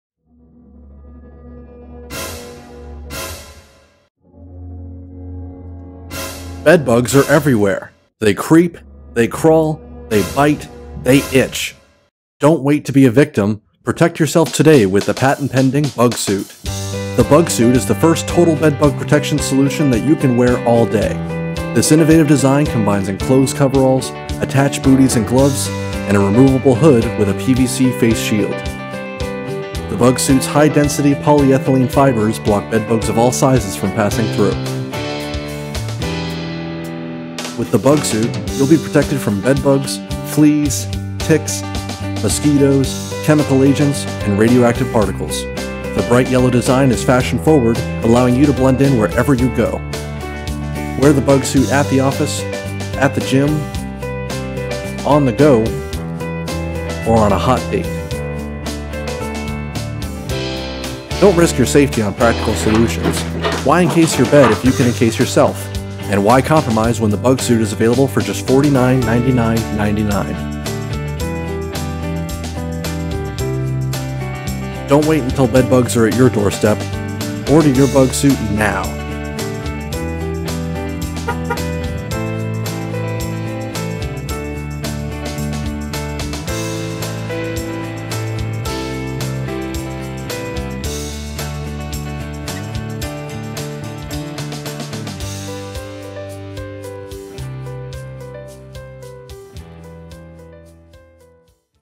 Introducing BUGSUIT, the 100% Bed Bug Proof Suit
Stop bed bugs from biting you by wearing a protective suit wherever you go! Introducing the BUGSUIT, the first 100% bed bug proof garment that you can wear all day